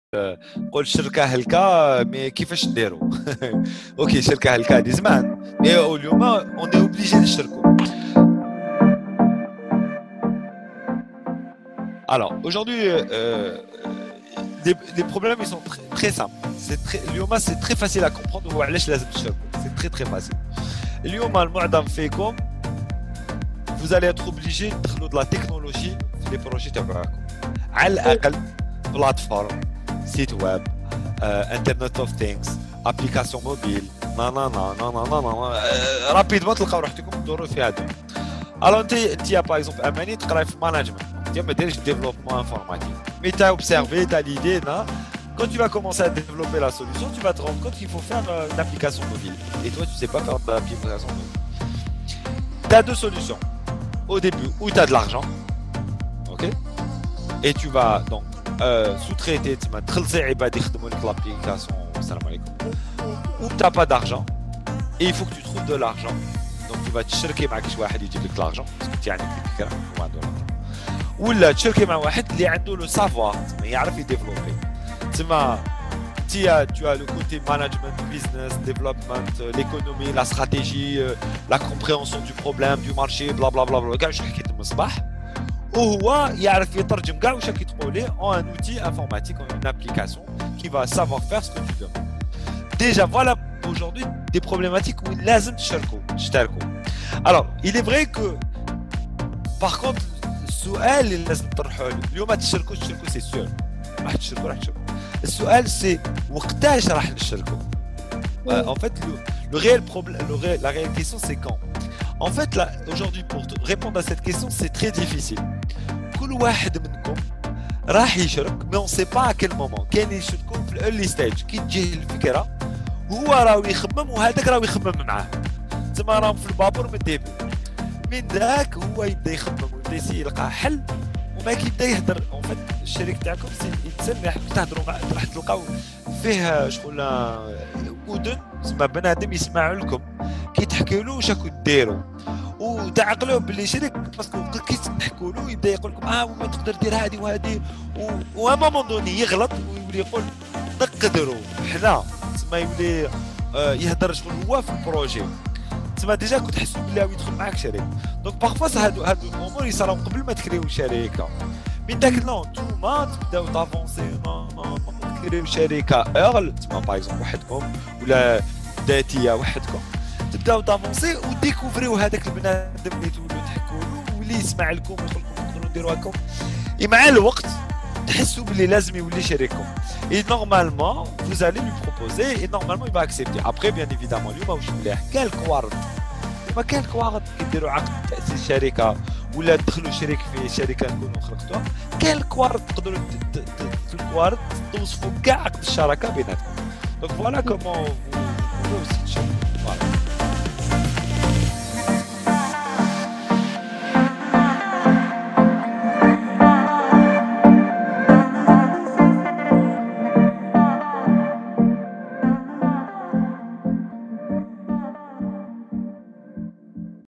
How to Choose your Business Partner ! E06S01
بزنس_قعدة مع كريم و اميرة !!
6th episode of business_kaada with karimbrouri & amirairmal , talking about How to Choose your Business Partner in business and enterpreneurship
Comment your questions below and tell us what do you think about the business ?